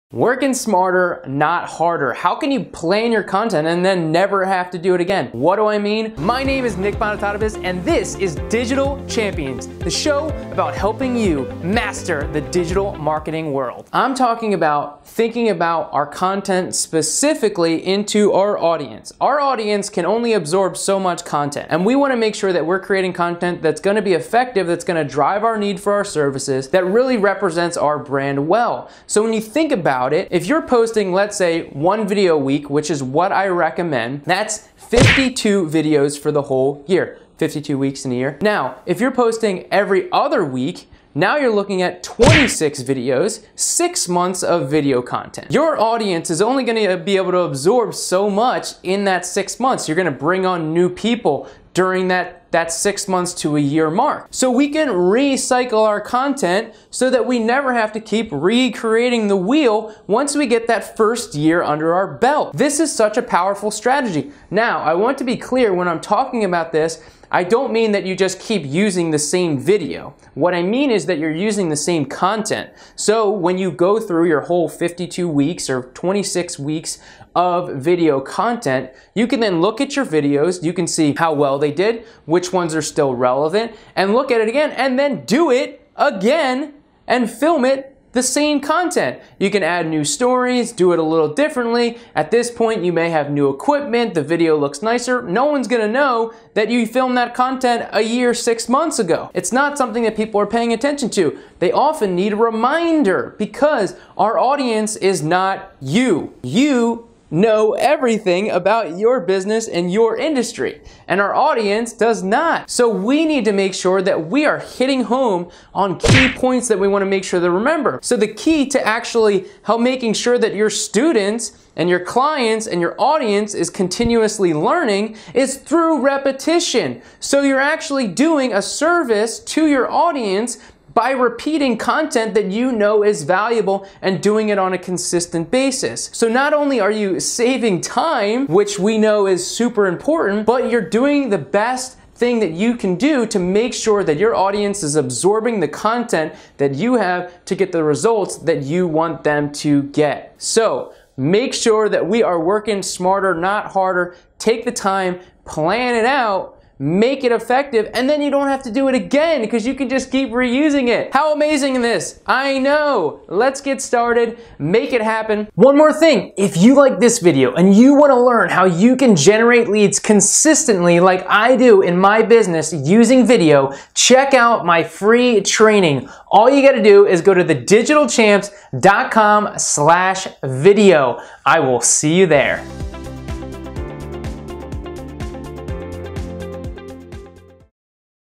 How To Plan Content AND Never Do It Again
Discover How To Create Effective Video That Generates Consistent Leads

-- LINKS --

-- FREE RESOURCES --

-- TODAY'S VIDEO --
As business owners, we have a lot of things to handle and one of those things is our content planning.

What if I told you there is a way to never plan your content again?

Don't believe me? Watch this video!